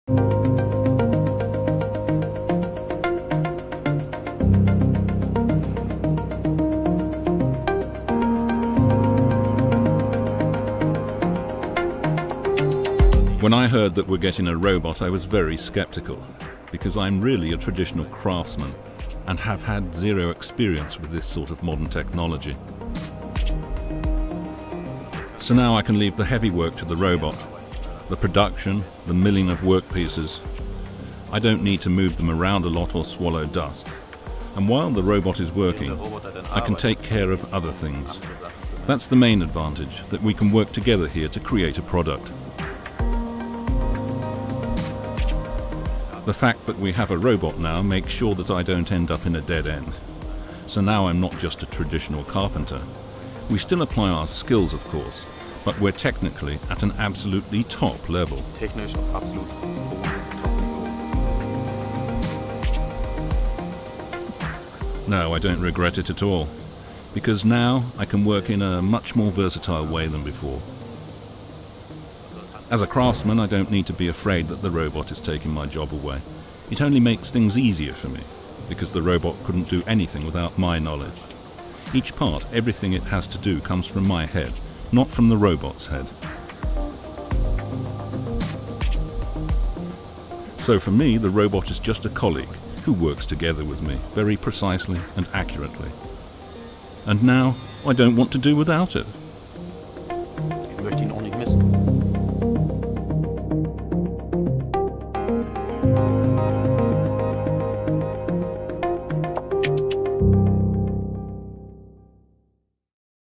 Workplace 4.0 | Part 2 - Robots in the carpentry shop
Traditional craftsmanship combined with high tech: Having a robot for a colleague makes more diverse activities and designs possible. The Future of Work at automatica 2023, June 27 - 30,  in Munich.